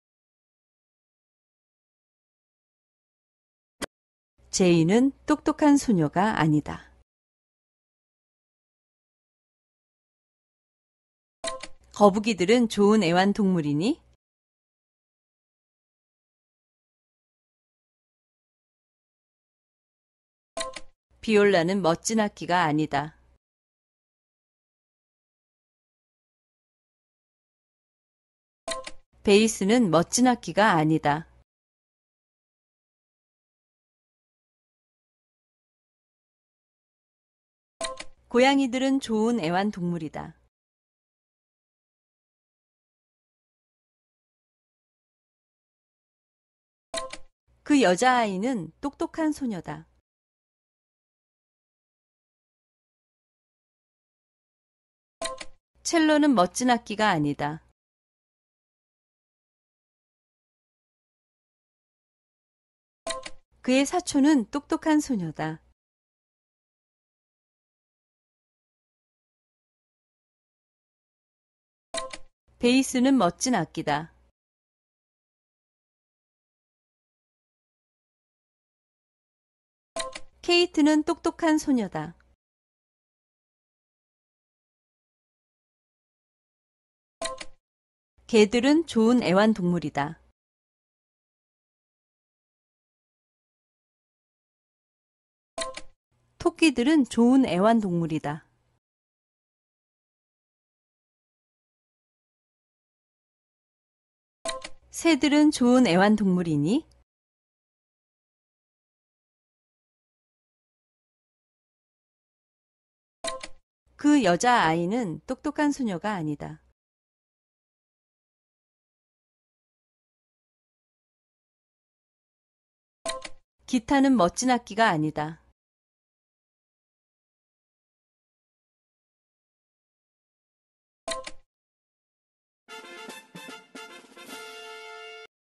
Test 5-1. ... is a smart girl
[ KEY ]
1. Jane is not a smart girl.
2. Are turtles nice pets?
3. The viola isn’t a beautiful instrument.
4. The base isn’t a beautiful instrument.
5. Cats are nice pets.
6. The girl is a smart girl.
7. The cello isn’t a beautiful instrument.
8. His cousin is a smart girl.
9. The base is a beautiful instrument.
10. Kate is a smart girl.
11. Dogs are nice pets.
12. Rabbits are nice pets.
13. Are birds nice pets?
14. The girl is not a smart girl.
15. The guitar isn’t a beautiful instrument.